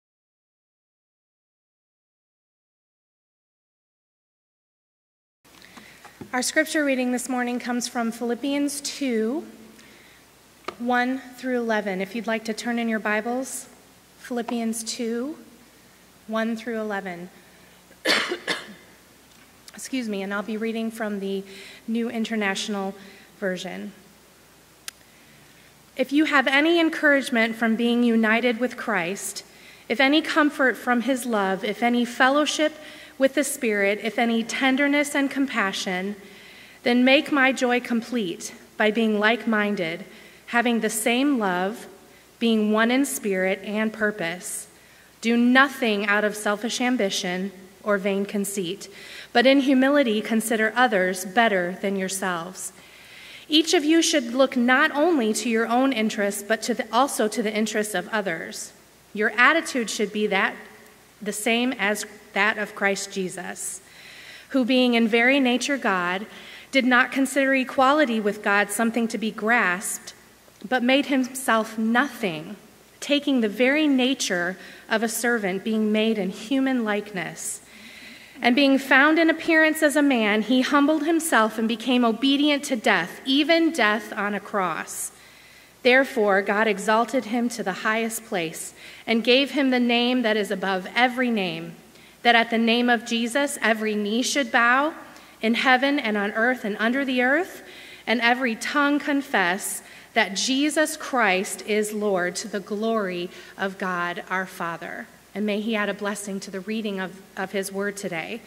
Scripture Philppians 2:1-11
Heather Hanton reads scripture (Philippians 2:1-11) during The Salvation Army Clearwater Citadel Holiness Meeting on 5/28/17